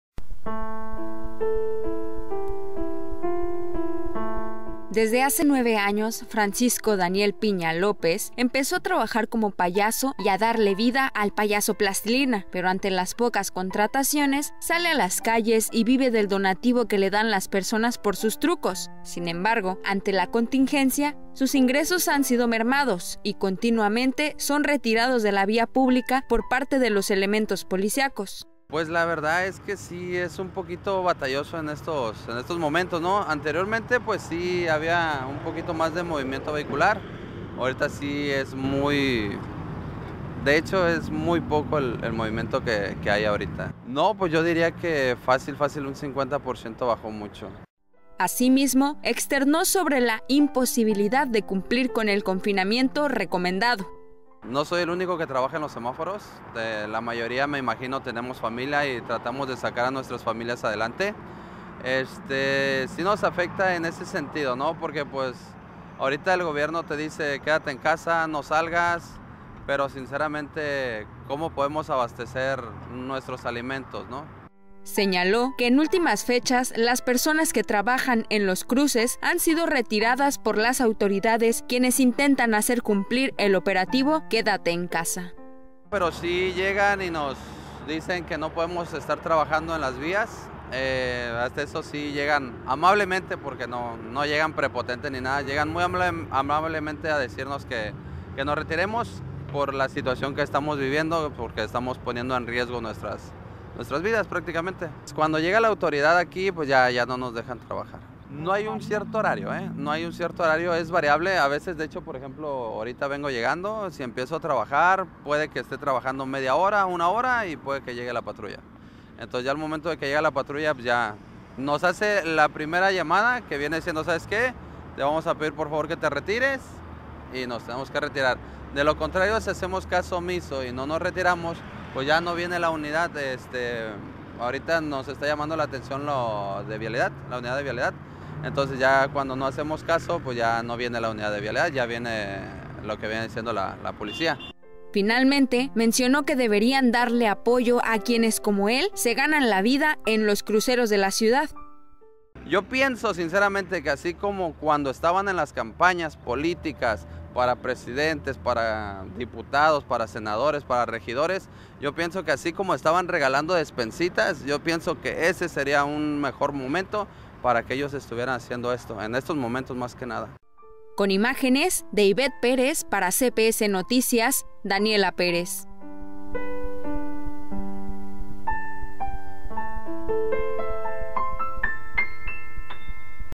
Payasos de cruceros piden que los dejen trabajar o les otorguen apoyos
Payasos de cruceros piden que los dejen trabajar o les otorguen apoyos para poder mantenerse durante la cuarentena.